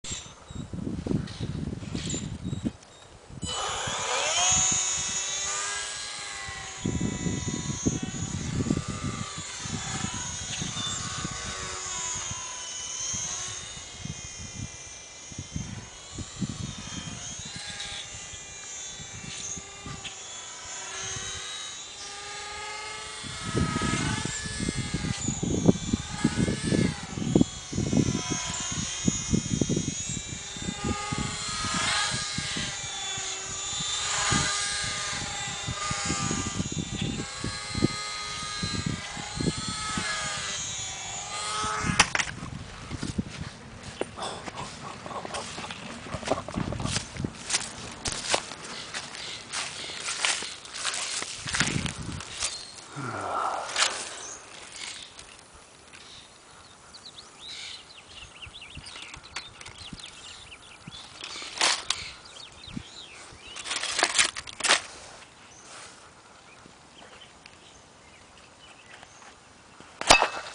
Sukhoi Su-26 scratchbuild micro RC plane crash
my scratch built micro Sukhoi Su-26, and its death.  Was powered with the ParkZone Vapor RX and a 7mm gearbox, 110mah lipo, running on ailerons and elevator...never even got a chance to install the rudder!